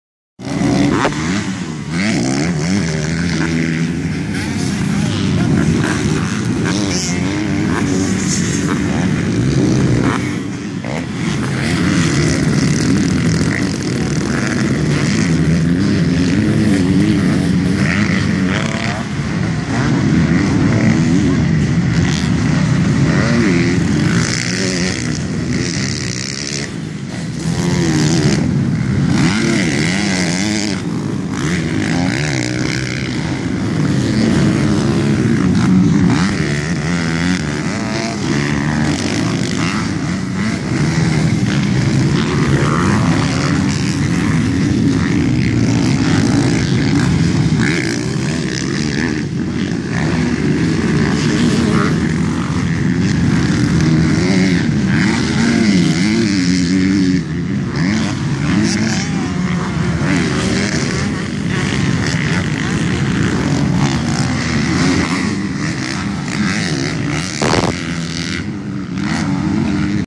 Raceway Park Motocross
Raceway Park Englishtown New Jersey Motocross Practice Track 1. Summer 2015